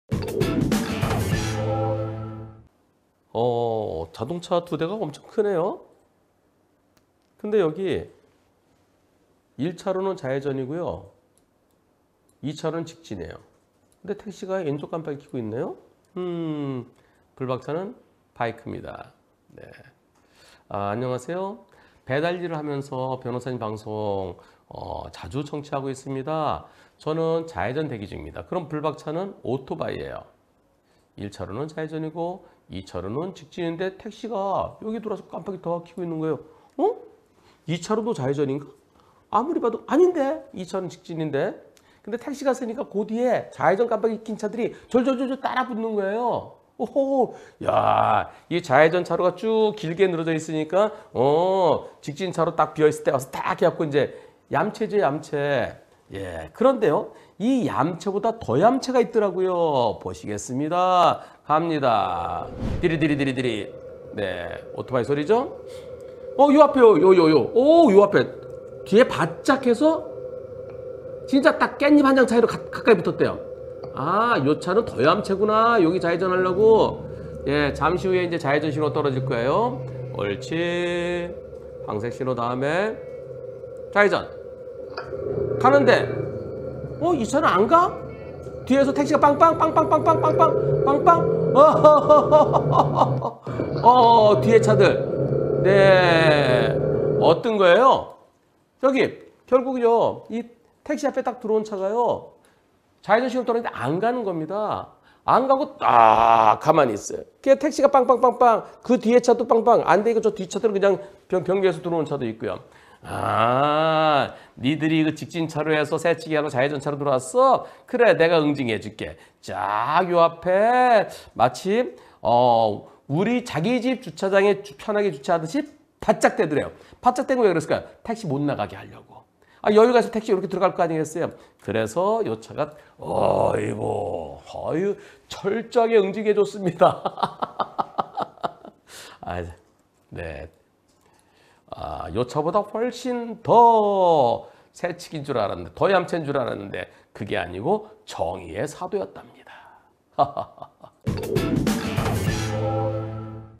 13139회. 정의의 사도(?) 아반떼
b19367,
211012 (화) 오전 생방송
맨 앞으로 끼어들어 얌체인줄 알았는데...

블박차는 오토바이, 교차로 1차로에서 좌회전 대기중에, 2차로에 좌측 깜빡이 켜고 대기중인 택시
그 뒤로도 좌회전 깜빡이 켜고 쭉 늘어섰는데, 잠시 후 2차로 맨 앞으로 차가 들어와 서서 서 있다가 신호가 바뀌었는데 멈춰버림.

뒤차들 좌회전 못하고 다 기다림

알고보니 정의의 사도

제가 좌회전 신호대기 중에 있는데요.

여기는 1차로만 촤회전 차선인데 2차선에 택시가 와서 좌회전 깜박이를 키고 아무렇지 않게 서 있는 거에요.

순간 제 눈을 의심하고 몇번이나 들여다 봤는데 거기는 직진차선이더라고요.

이후 택시 뒤로 차들이 쭉 줄을 서는 거에요.

그러다 구)아반떼가 3차선으로 달리다 택시 앞에 서더니 자기집 주차하는 거 마냥 택시 앞을 정밀하게 후진시켜 5센치 이내로 주차를 합니다.

그리고 좌회전이 되자 1차선 차들은 신호 맞춰 가는데 2차선 아반떼는 가질 않고 택시는 크락션을 수회 누르더라구여.

저는 그때 알았습니다.

아반떼가 좌회전을 목적으로 거기에 온 게 아니라는 것을요ㅎㅎ

한문철 변호사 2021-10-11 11:30
2차로는 직진 차로인데

1차로에 좌회전 대기중인 차들이 줄지어 서 있으니

택시가 얌체처럼 2차로에서 좌회전하려고 대기 중이고

그 뒤에도 좌회전하려는 차들이 줄지어 서고

그러던 중 택시 앞으로 차 한 대 들어오기에 그 차도 좌회전하려나 했는데

그 차는 좌회전이 아닌 직진하려는 차였군요.

그 차가 얌체같이 좌회전하려는 차들을 응징한 거였군요~ 맞나요? ㅎㅎ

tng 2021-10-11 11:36
네 맞습니다.

아반떼가 2차선으로 오다가 2차선이 막혀있으니 3차선으로 변경 후 2차선으로 들어와서 진입후 가정주차하듯 하고 버티게 된겁니다. 아반떼는 2차선에서 올때 하이빔두 쐈습니다.